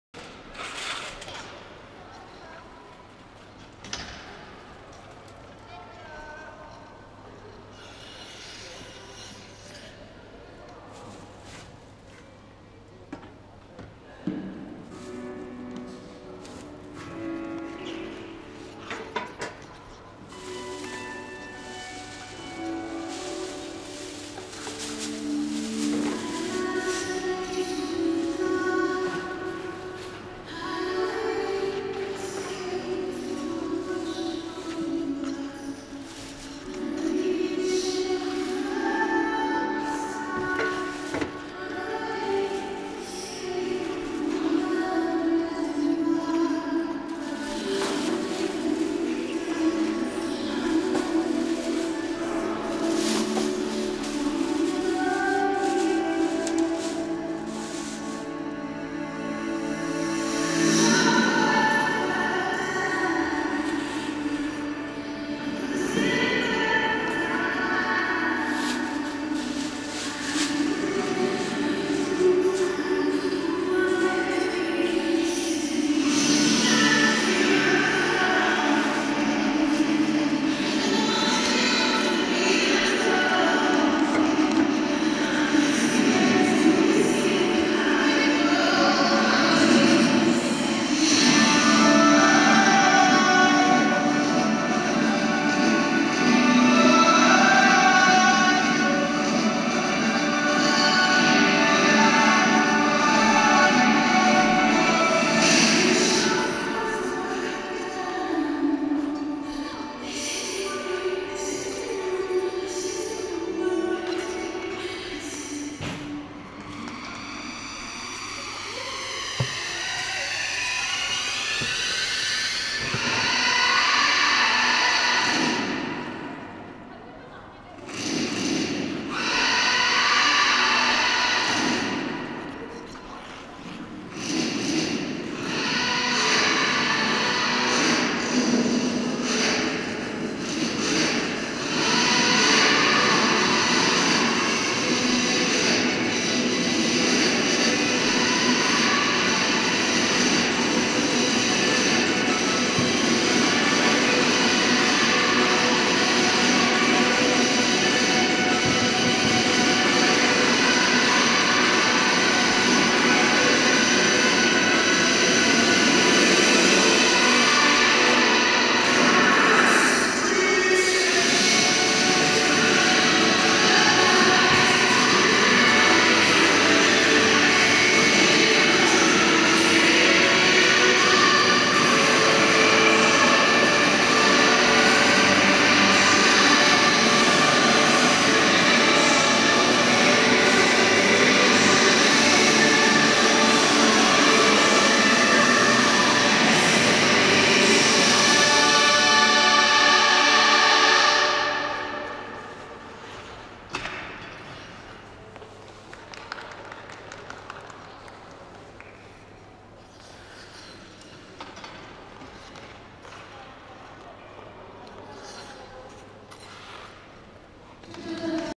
여자4급 초등부4학년 FS 손가인,서울목운초4 @ 2017년 서울시회장배 생활체육빙상대회
2017/12/16, 목동 아이스링크, 2017년 서울특별시빙상경기연맹 회장배 생활체육 빙상대회